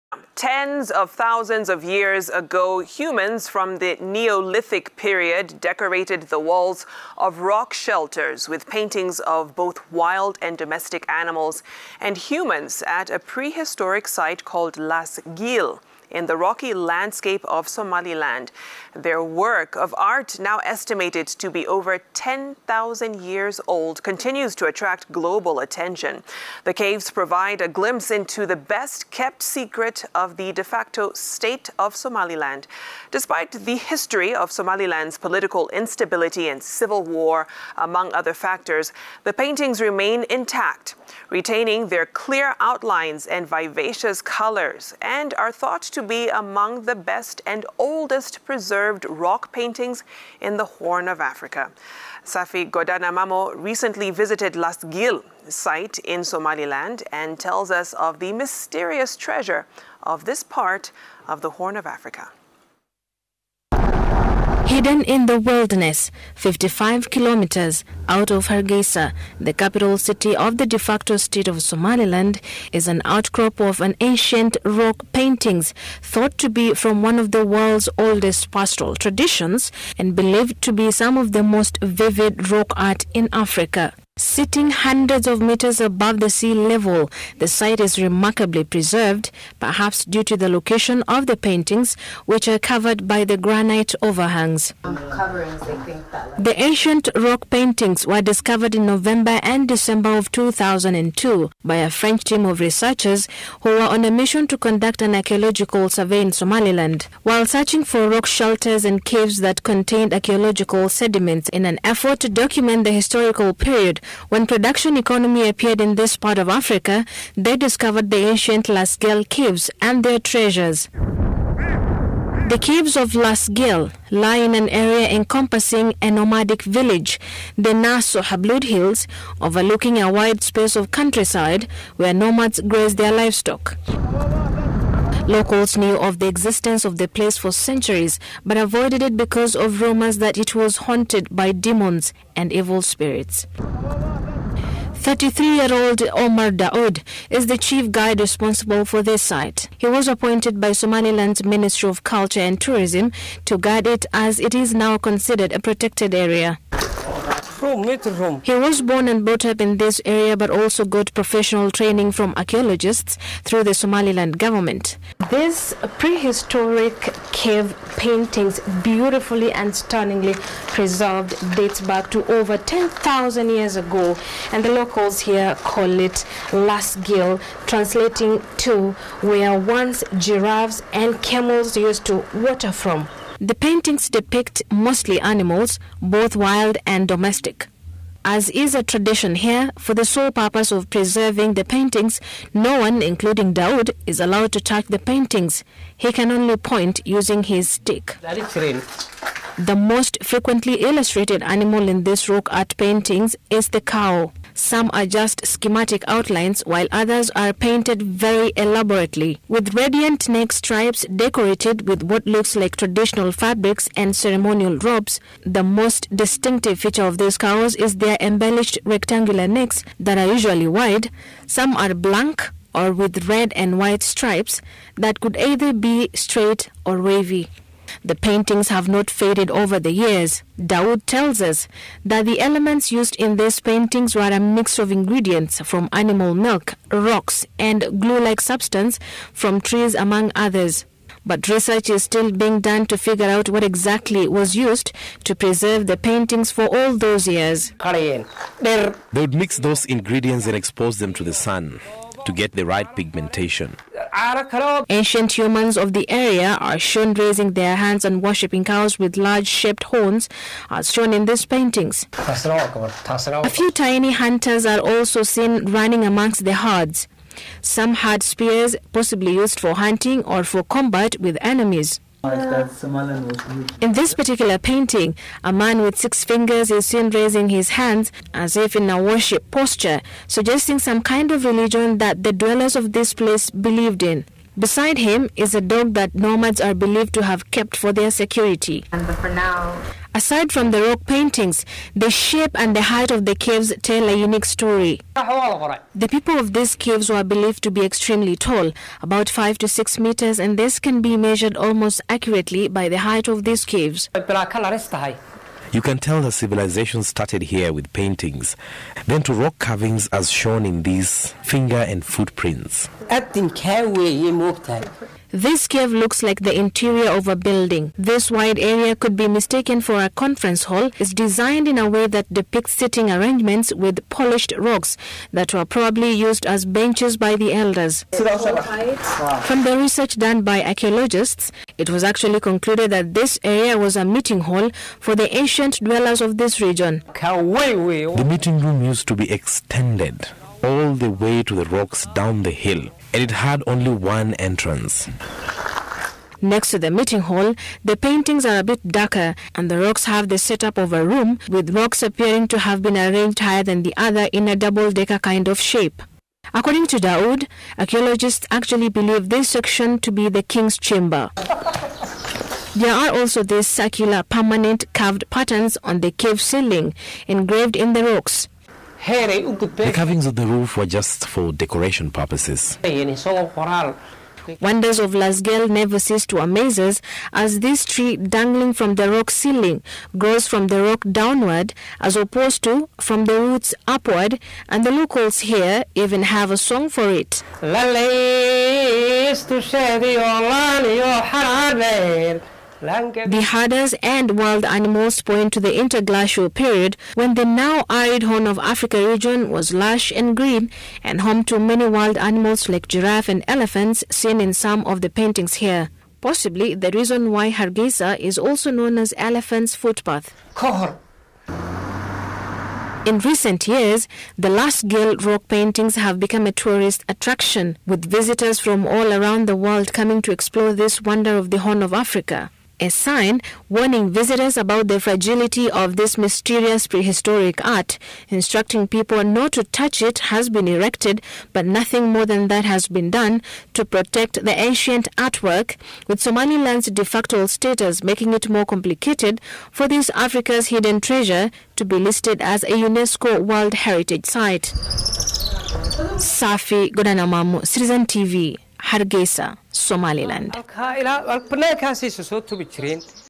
Paintings in the Laas Geel caves is estimate to be over 10,000 years
Tens of thousands of years ago, humans from the Neolithic period, decorated the walls of rock shelters with paintings of both wild and domestic animals and humans at a pre -historic site called Laas Geel in the rocky landscape of Somaliland. Their work of art, now estimated to be over 10,000 years old continues to attract global attention. The caves provide a glimpse into the best kept secret of the de-facto state of Somaliland.
Despite the history of Somaliland’s political instability and civil war among other factors, the paintings remain intact, retaining their clear outlines and vivacious colors and are thought to be among the best and oldest preserved rock paintings in the horn of Africa. Safi Godana Mamo recently visited Laas Geel site in Somaliland and tells us of the mysterious treasure of this part of Horn of Africa.